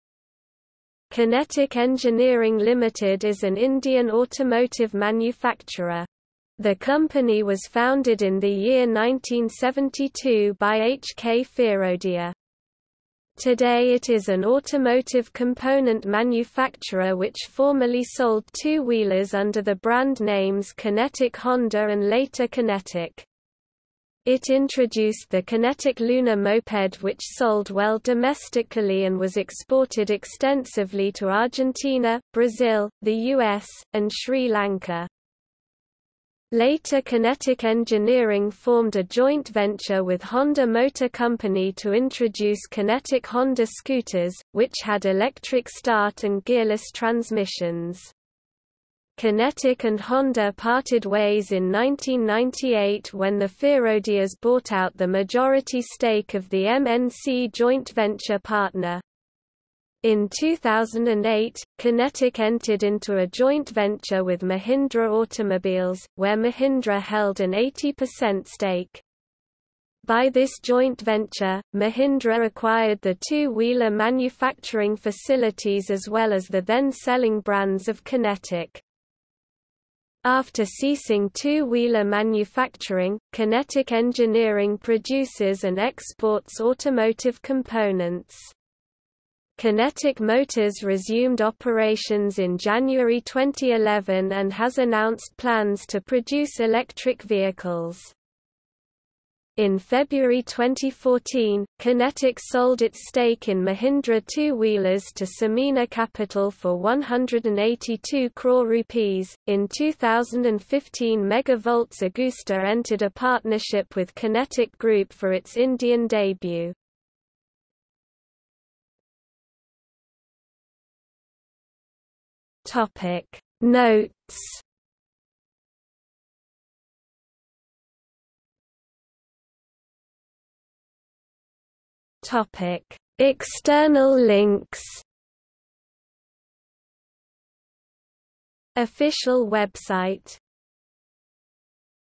Kinetic Engineering Limited | Wikipedia audio article
00:01:59 Notes
00:02:08 External links

- Socrates

SUMMARY
Kinetic Engineering Limited is an Indian automotive manufacturer. The company was founded in the year 1972 by HK Firodia. Today it is an automotive component manufacturer which formerly sold two-wheelers under the brand names Kinetic Honda and later Kinetic. It introduced the Kinetic Luna moped which sold well domestically and was exported extensively to Argentina, Brazil, the US, and Sri Lanka.
Later Kinetic Engineering formed a joint venture with Honda Motor Company to introduce Kinetic Honda scooters, which had electric start and gearless transmissions. Kinetic and Honda parted ways in 1998 when the Firodias bought out the majority stake of the MNC joint venture partner. In 2008, Kinetic entered into a joint venture with Mahindra Automobiles, where Mahindra held an 80% stake. By this joint venture, Mahindra acquired the two-wheeler manufacturing facilities as well as the then selling brands of Kinetic.
After ceasing two-wheeler manufacturing, Kinetic Engineering produces and exports automotive components. Kinetic Motors resumed operations in January 2011 and has announced plans to produce electric vehicles.
In February 2014, Kinetic sold its stake in Mahindra Two-Wheelers to Samena Capital for 182 crore rupees.In 2015 MV Agusta entered a partnership with Kinetic Group for its Indian debut.